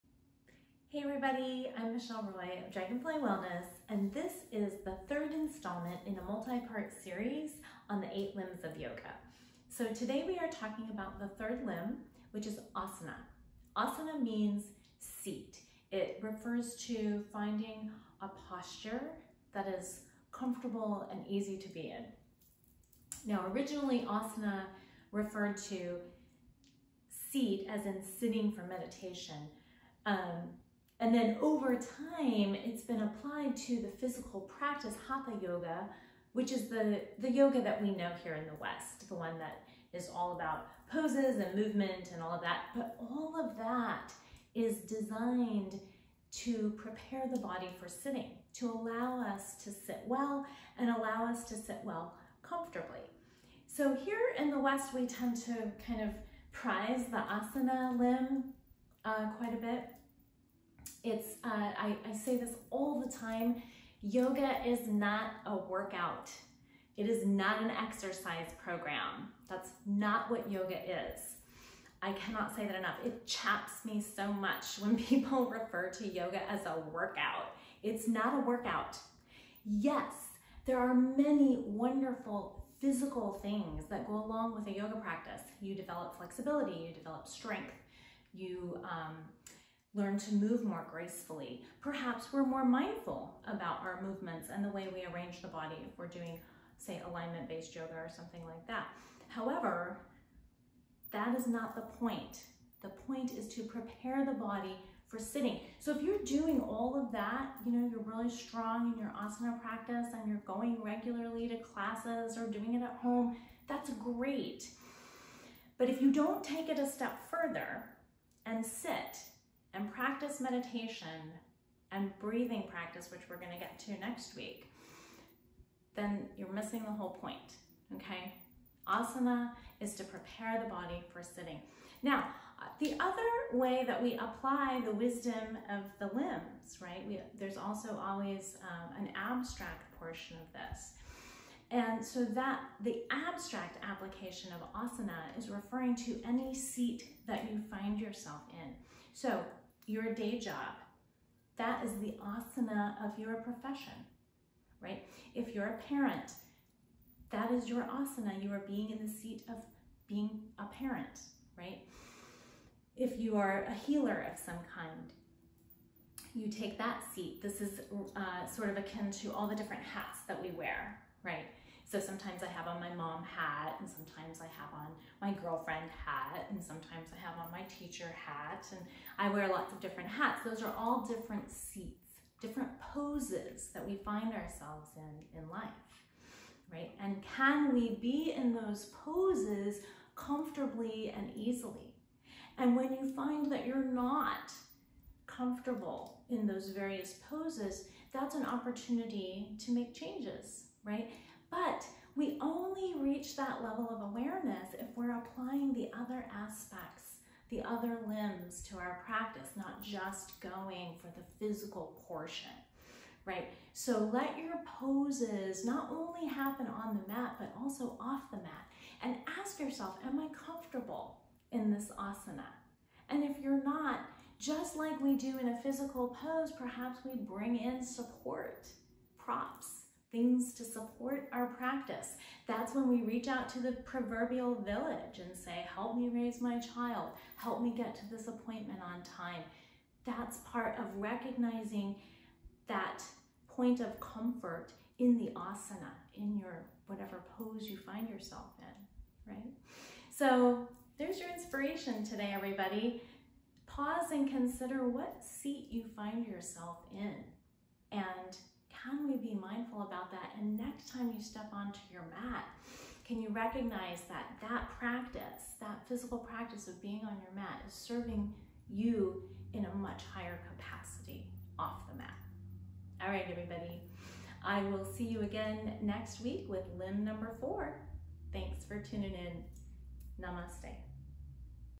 Thoughtful Thursday: Asana, The Third Limb of Yoga | Michelle Roy at Dragonfly Wellness
Hello again! This is part three of a multi-part series on the 8 limbs of yoga. :)

Visit our websites to learn more about our classes, read our blog and shop our apothecary 💜